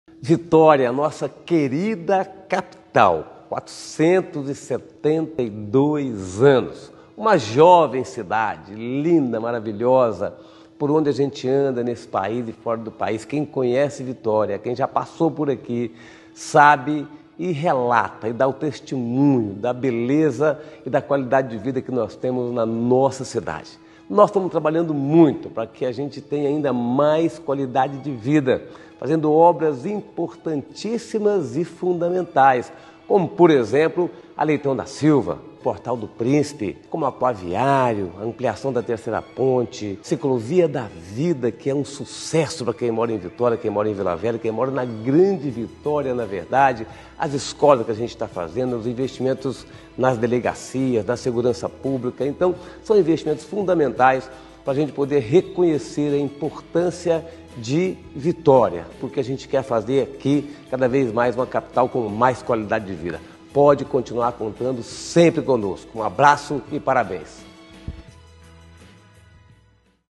Vitória 472 anos: Uma história de tradição, belezas e encantamentos
472 anos de muita história, beleza, tradição e encantamento. A nossa Capital Vitória está em festa hoje e nós temos muita alegria de, ao longo dos últimos anos, trazer investimentos para melhorar a vida das pessoas que moram e circulam por aqui!